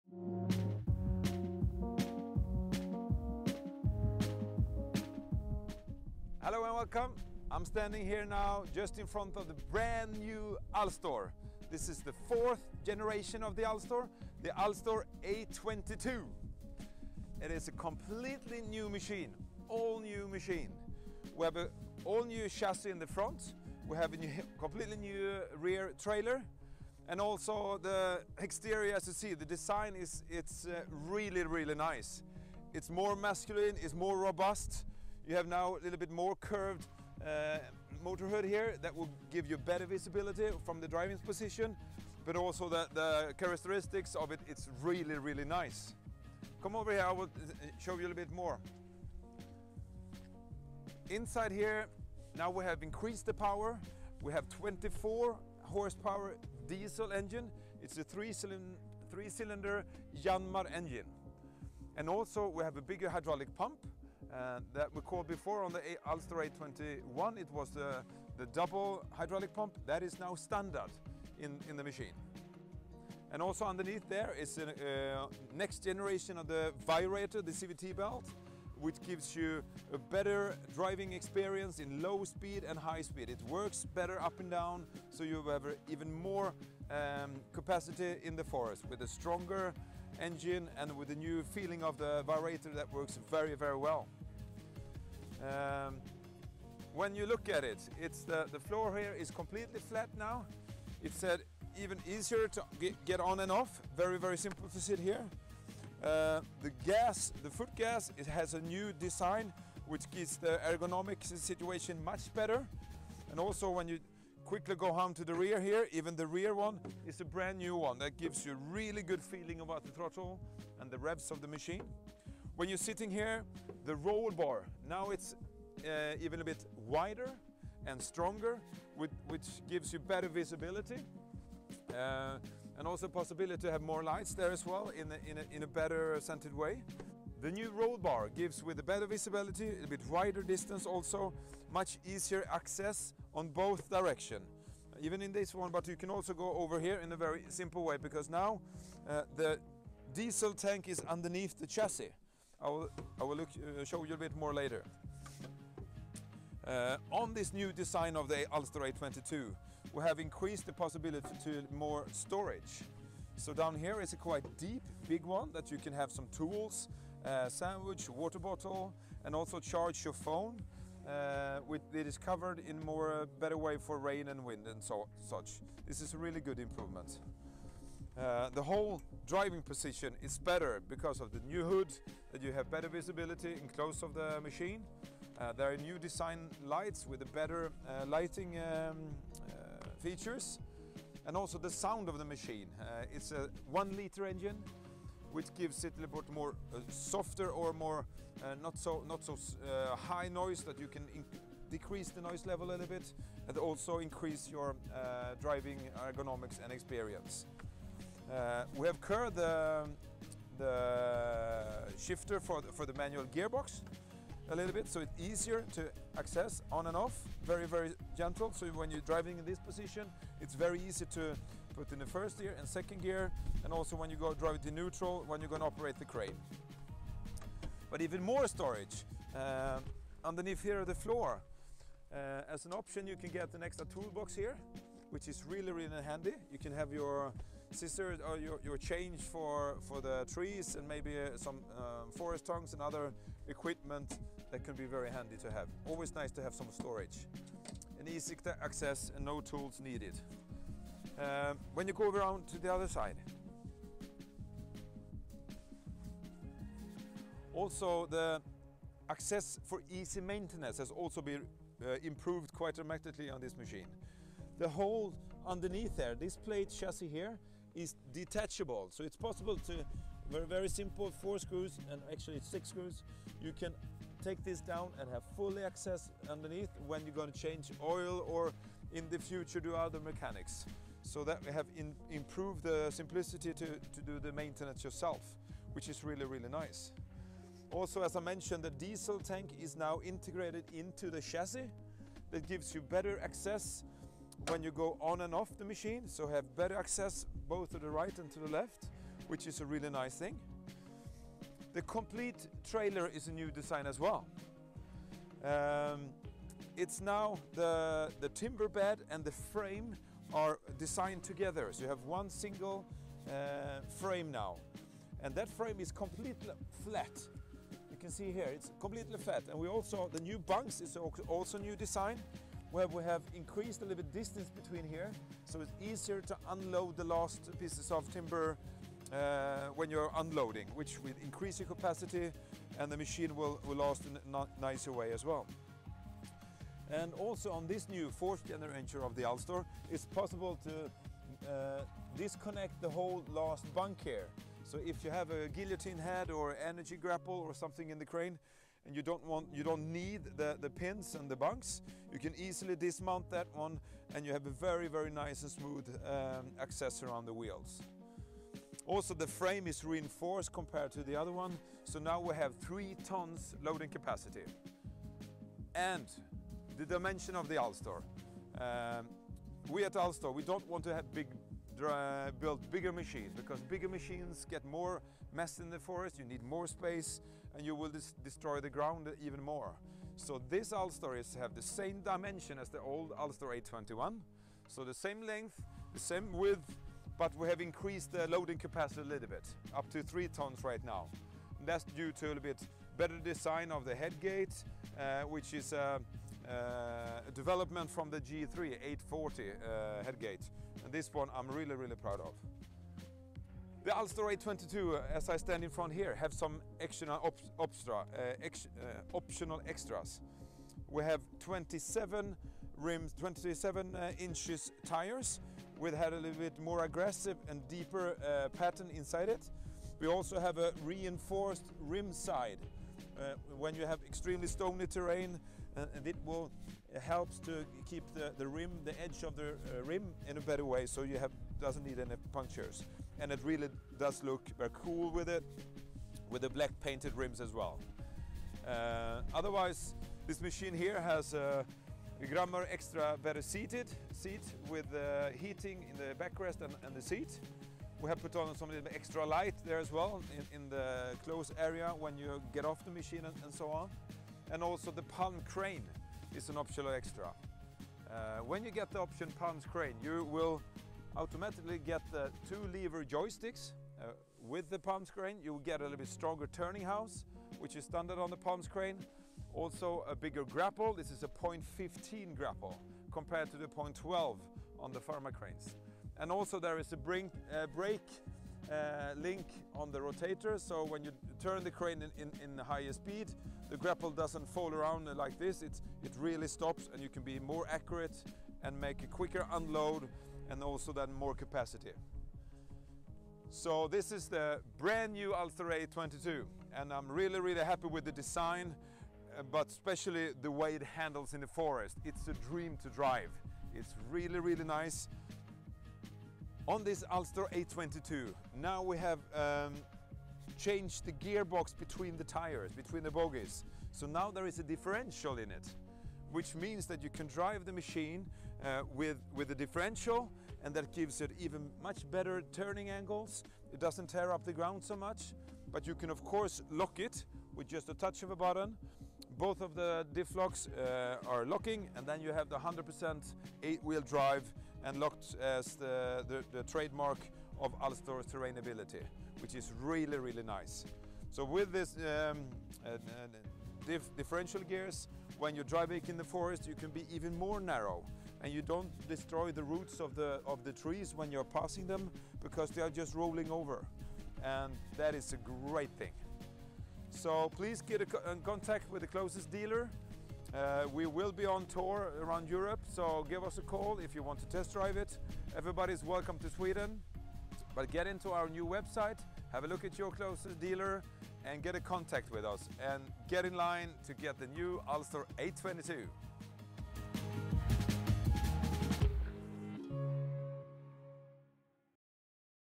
The all new Alstor 822.
The 4th generation of Alstor 8x8 is now here.
Alstor 822!

For more info please contact your nearest dealer.